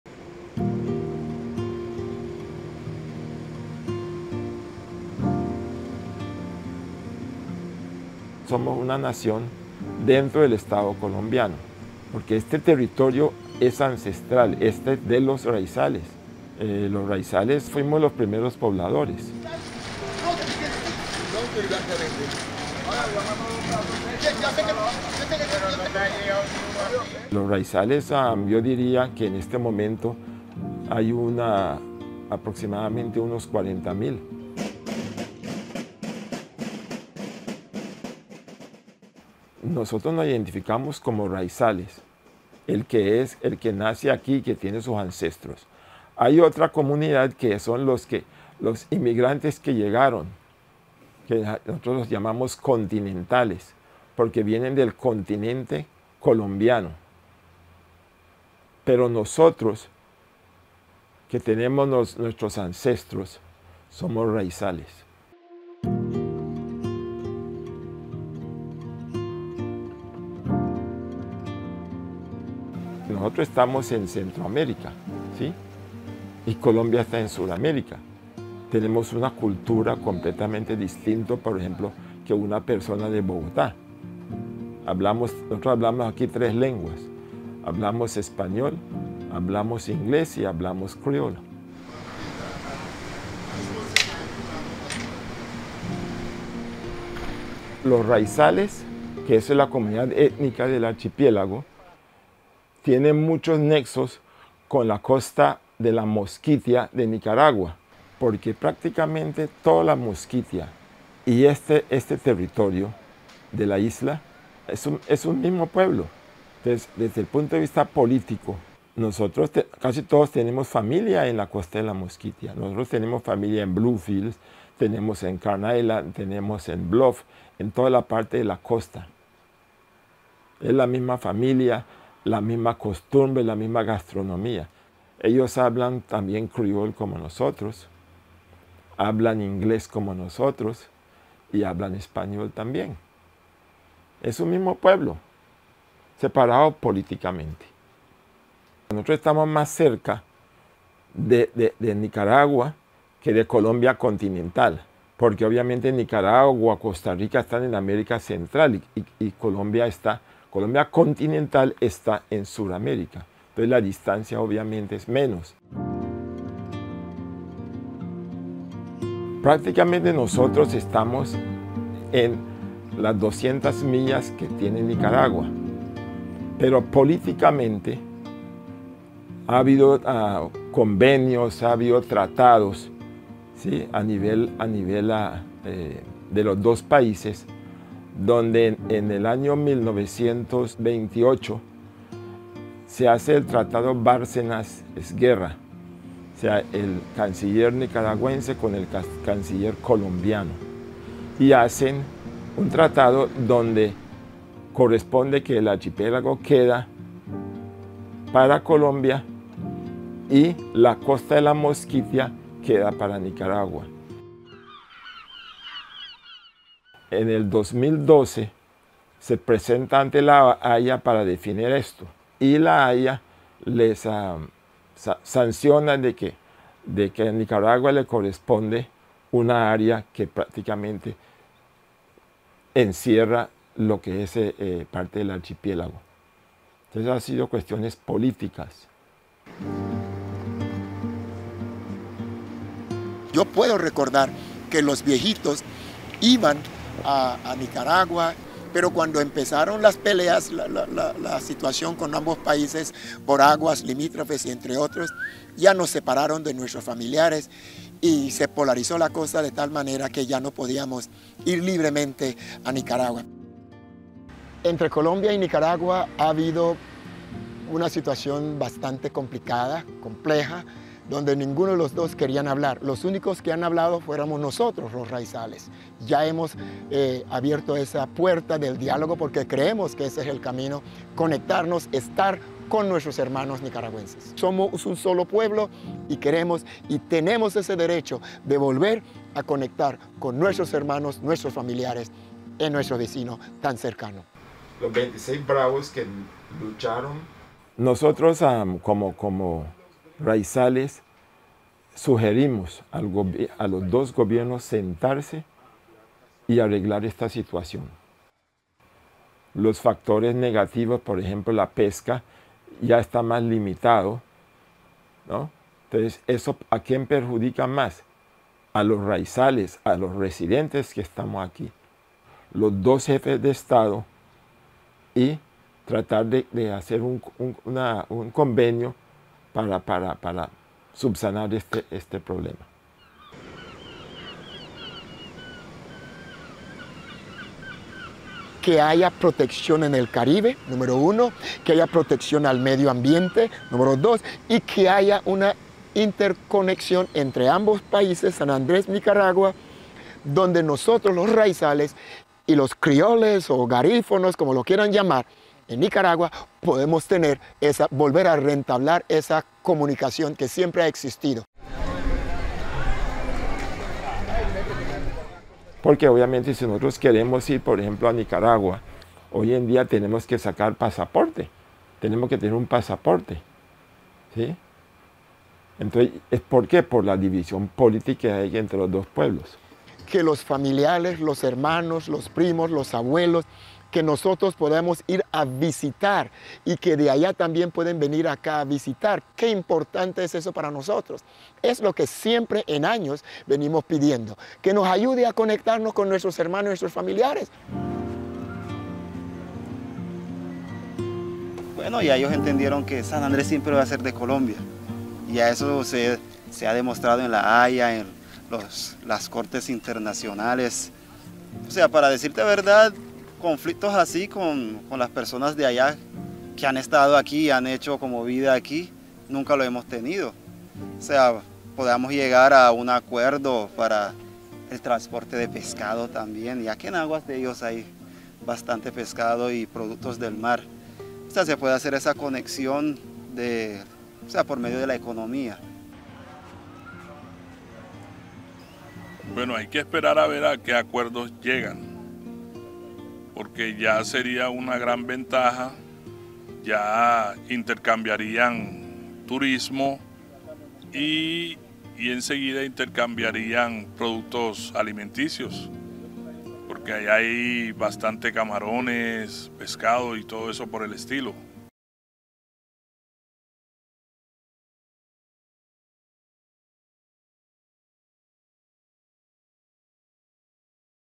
Parte 1 | San Andrés, un paraíso en disputa entre Nicaragua y Colombia
Nicaragua y Colombia se han disputado durante casi un siglo la soberanía del archipiélago de San Andrés, ubicado en el Mar Caribe. Aunque el diferendo ha generado conflicto entre ambos gobiernos, sus pobladores se sienten como un mismo pueblo separado políticamente